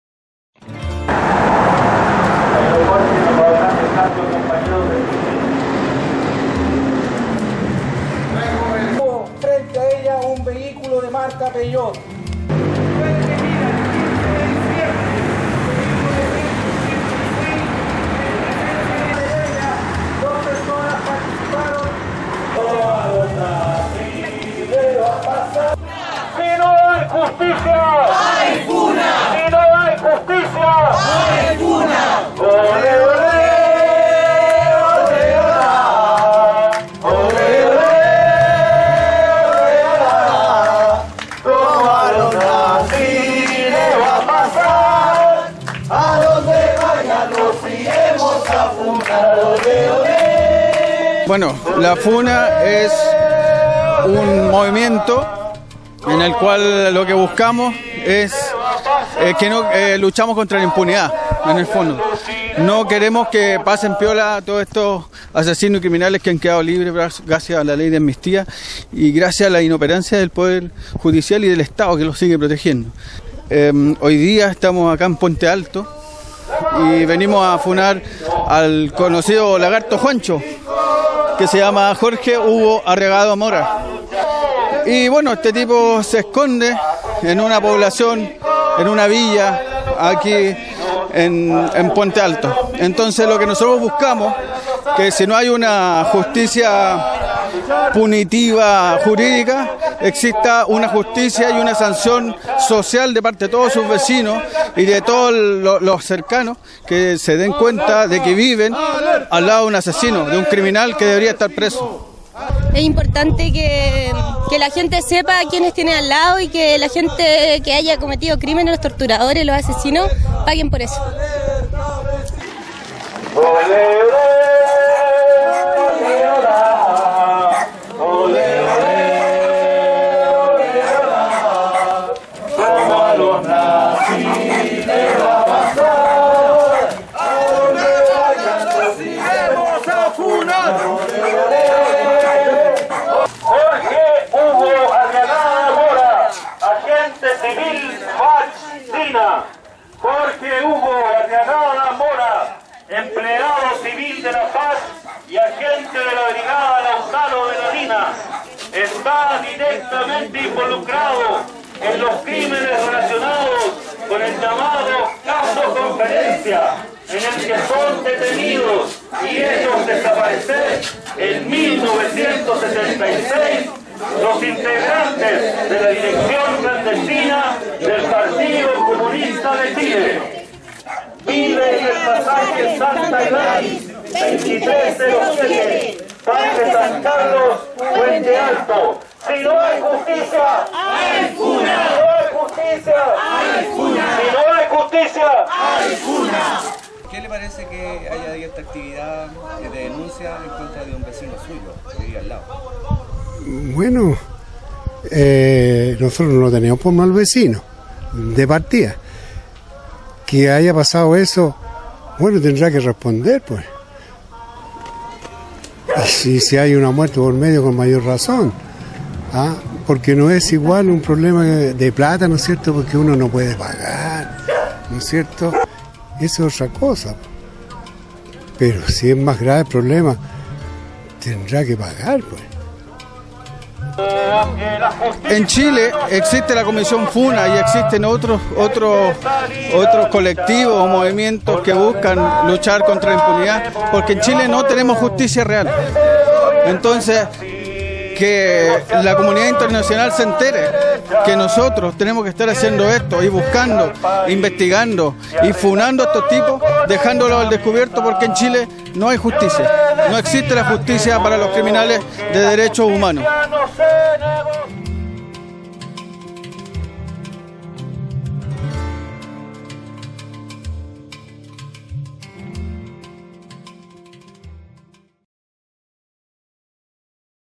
IMPUNIDAD EN CHILE: SI NO HAY JUSTICIA, HAY FUNA
En Chile, solo 177 ex agentes represivos de la Dictadura cívico-militar cumplen presidio efectivo, condenados por violaciones a los Derechos Humanos, según el balance entregado por el programa de DDHH, del Ministerio del Interior, el pasado 10 de diciembre.
El reciente sobreseimiento del ex militar Sergio Arellano Stark, uno de los máximos responsables de la “Caravana de la Muerte”, que recorrió el país asesinando opositores al régimen de Pinochet; La sanción de solo 400 días de reclusión dictada por la Corte Suprema, al sargento de carabineros Miguel Millacura, como autor de la muerte del joven Manuel Gutiérrez; ocurrida en 2011, los “pactos de silencio” de las fuerzas armadas; la aplicación de leyes de amnistía, y el lento accionar de los tribunales por los crímenes de lesa humanidad, que ocurren en el país, entre otros. Configuran una imagen de impunidad, por la falta de voluntad política por resolver la deuda pendiente del Estado, con las víctimas y la sociedad, en materia de DDHH.
Ante la denegación de justicia, surgen movimientos como la FUNA, que a través de la denuncia, intentan desmontar esta impunidad y van por los represores a sus barrios, lugares de trabajo, etc. Donde los exponen ante la comunidad, para dejar en claro el pasado de estos ex agentes, como un ejercicio de justicia social.
Hoy sábado el movimiento FUNA, denunció a un ex agente de la policía secreta de Pinochet (DINA) en su domicilio, ubicado en la comuna de Puente, de Santiago. Se trata de Jorge Arriagada, ex miembro de la brigada especial de extermio “Lautaro” de la DINA, responsable del asesinato de miles de chilenos.
Bajo la consigna "Si no hay justicia, hay funa”, una centena de manifestantes recorrió las calles de la villa “Parque San Carlos”, desenmascarando el pasado de Arriagada.
La Dirección de Inteligencia Nacional (DINA), era dirigida por militares, formados en la Escuela de las Américas del Ejército de EEUU.